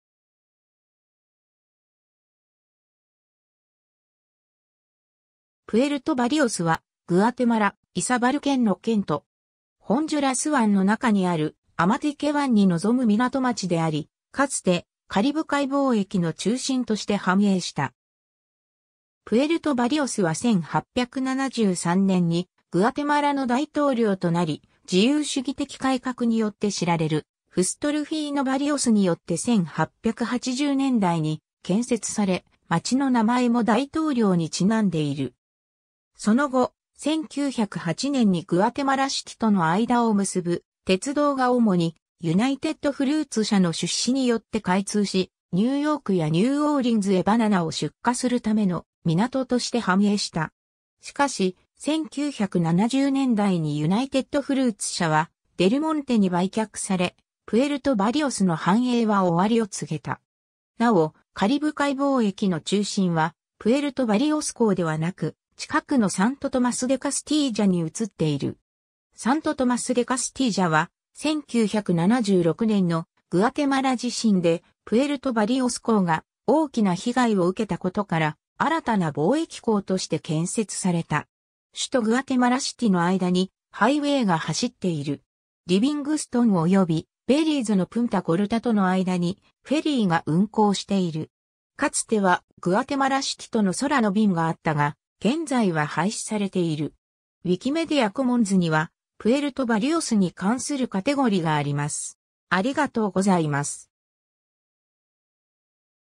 プエルト・バリオス
プエルト・バリオス（Puerto Barrios）は、グアテマラ、イサバル県の県都。
ホンジュラス湾の中にあるアマティケ湾に臨む港町であり、かつてカリブ海貿易の中心として繁栄した。
 プエルト・バリオスは1873年にグアテマラの大統領となり、自由主義的改革によって知られるフスト・ルフィーノ・バリオスによって1880年代に建設され、町の名前も大統領にちなんでいる。
その後、1908年にグアテマラシティとの間を結ぶ鉄道が主にユナイテッド・フルーツ社の出資によって開通し、ニューヨークやニューオーリンズへバナナを出荷するための港として繁栄した。
しかし、1970年代にユナイテッド・フルーツ社はデルモンテに売却され、プエルト・バリオスの繁栄は終わりを告げた。
 なお、カリブ海貿易の中心はプエルト・バリオス港ではなく、近くのサント・トマス・デ・カスティージャに移っている。
サント・トマス・デ・カスティージャは、1976年のグアテマラ地震でプエルト・バリオス港が大きな被害を受けたことから、新たな貿易港として建設された。
 首都グアテマラシティの間にハイウェイ(CA-9)が走っている。
 リビングストンおよびベリーズのプンタ・ゴルタとの間にフェリーが運航している。
 かつてはグアテマラシティとの空の便があったが、現在は廃止されている。
 ウィキメディア・コモンズには、プエルト・バリオスに関するカテゴリがあります。